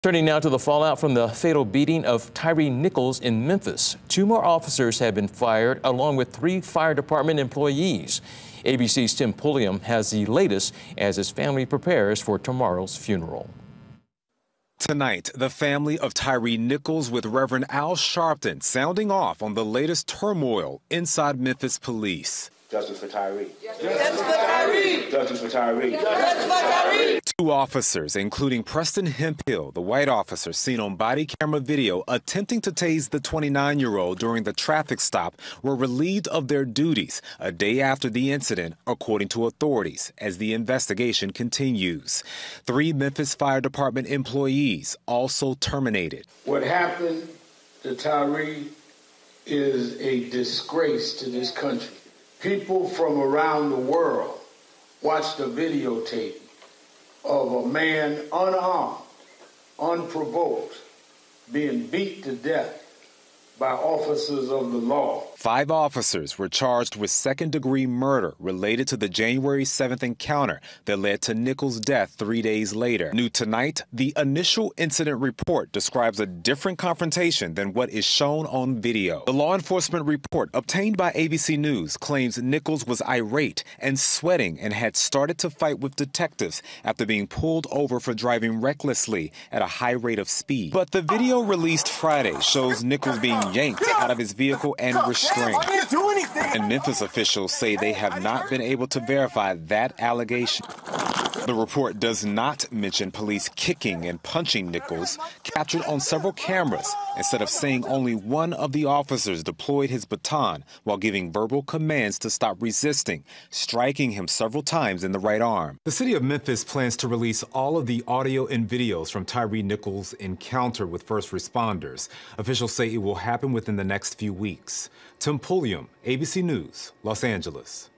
More officers fired in Tyre Nichols death
Two more officers have been fired along with three fire department employees.